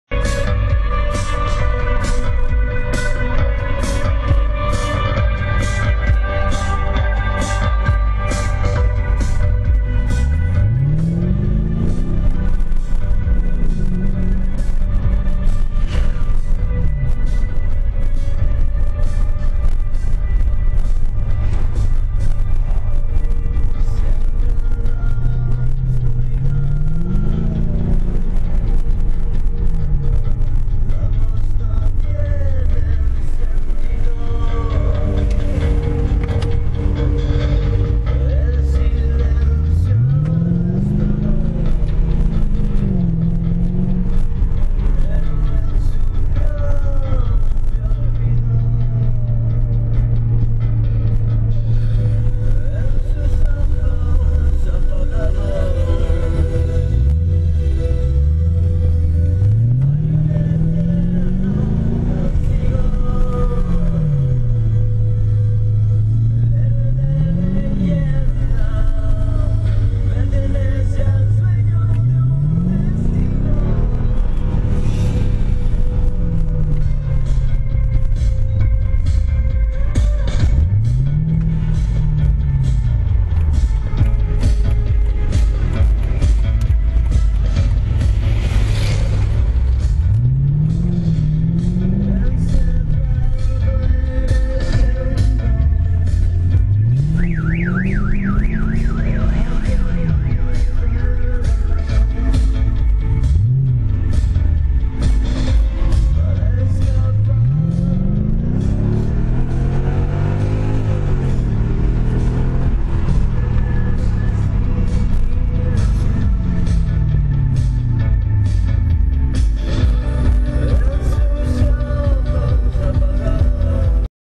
Mercedes Benz C55 AMG with Kleemann S7 notes activating car alarms.
Kleemann performance products for Mercedes-Benz
cars offer unmatched performance, exclusive design
and factory-like reliability, some products may cause that others cars alarms could be activated by the roar of the exhaust as shown in this video.